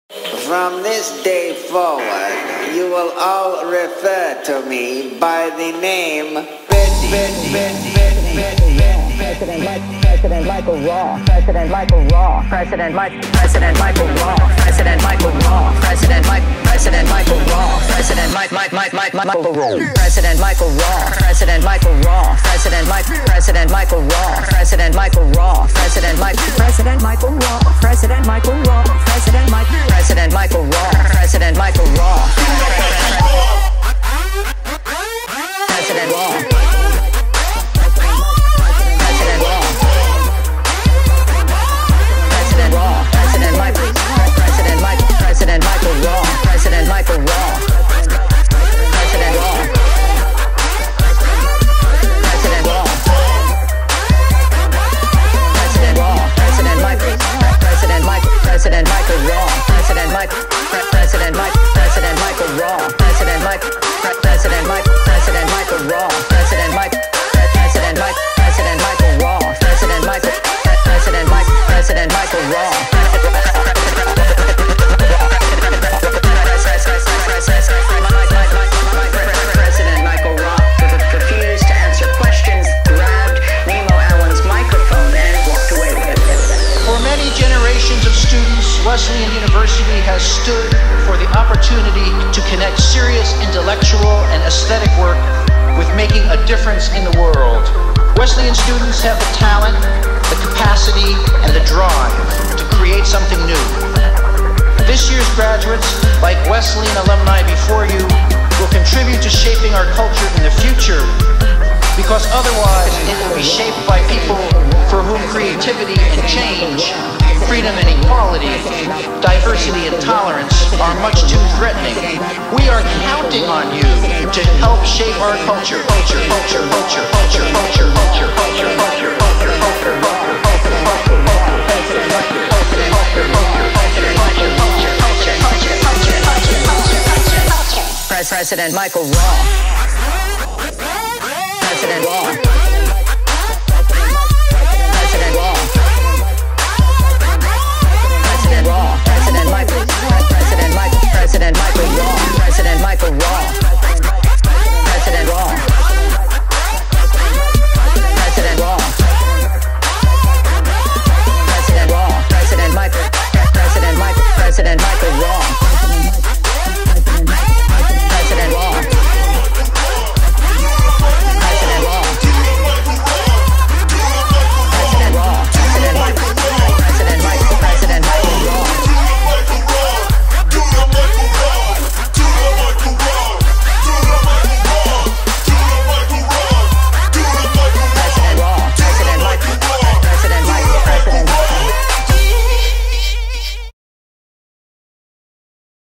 How To Do The Michael Roth
Instructional Video on how to the the Michael Roth

Video: Dan Wissinger
Choreography: Michael Roth
Editing: Betty

Long Live MR!